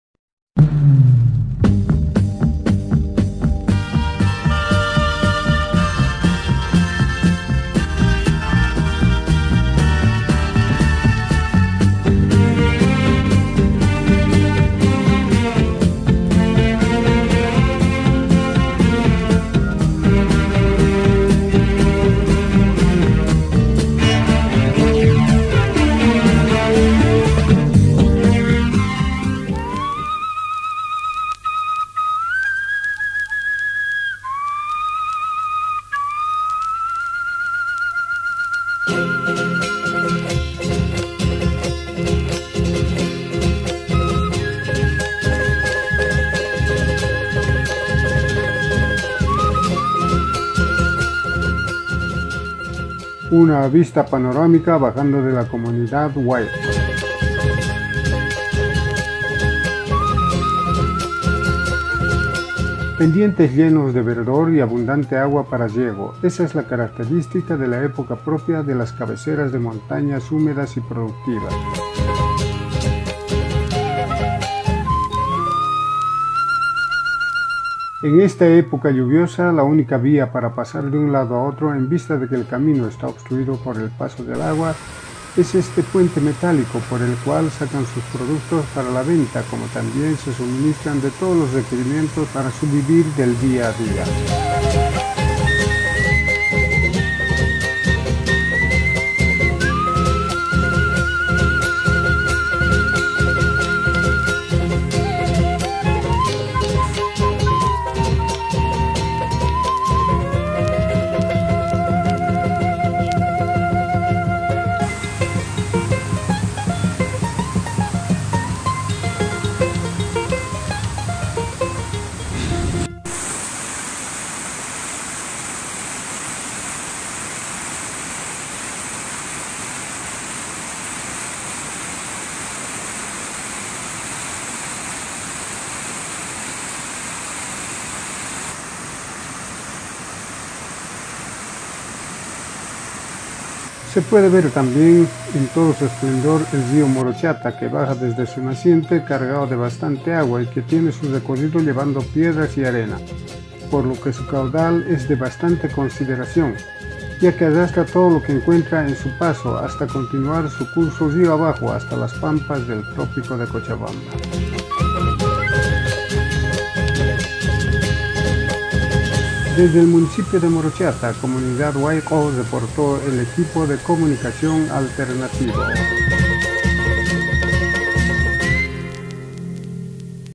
Documental desde Wayqo
Wayqo, comunidad del Municipio de Morochata - Cochabamba - Bolivia, paseando contemplando sus hermoso paisajes